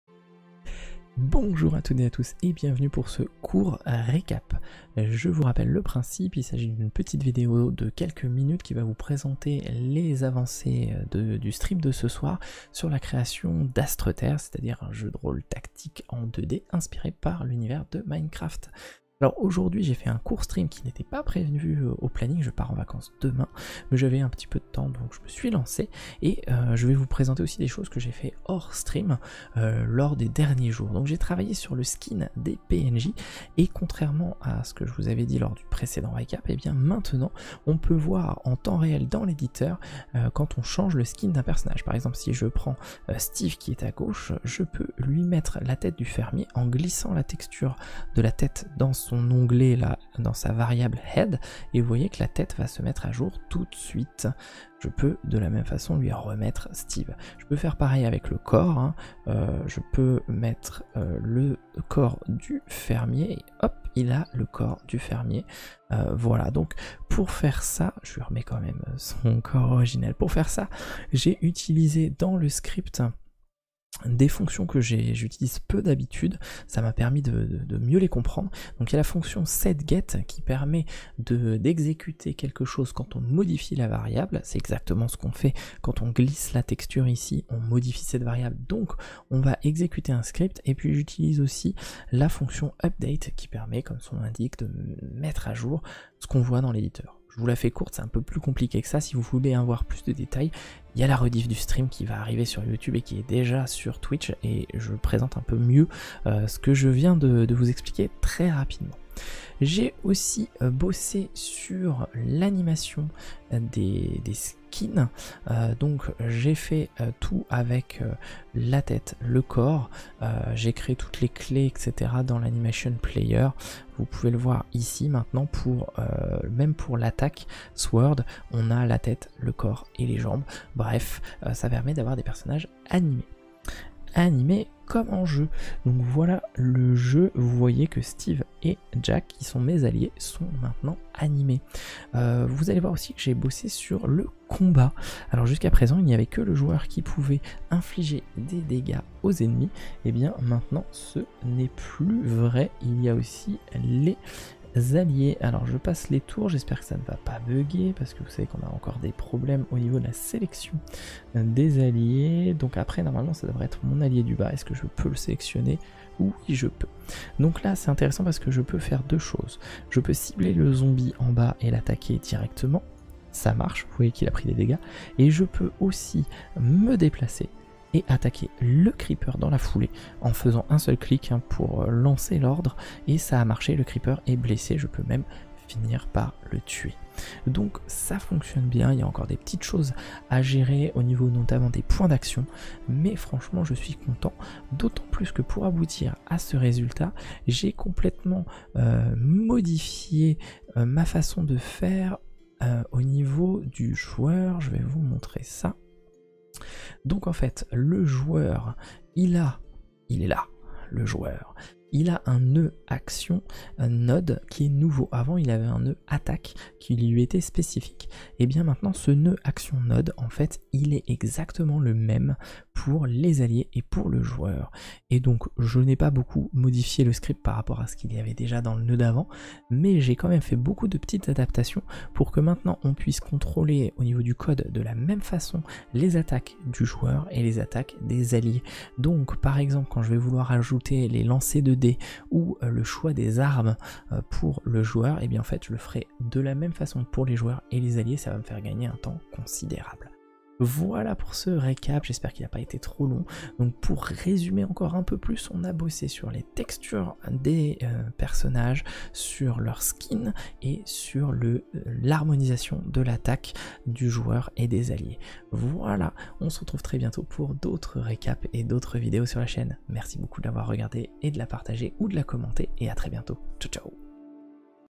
Astreterre // Récap' #2 - Skin & Attaque des alliés !
Petite vidéo récapitulative de ce qu'on a fait lors du stream du 19/08/22 !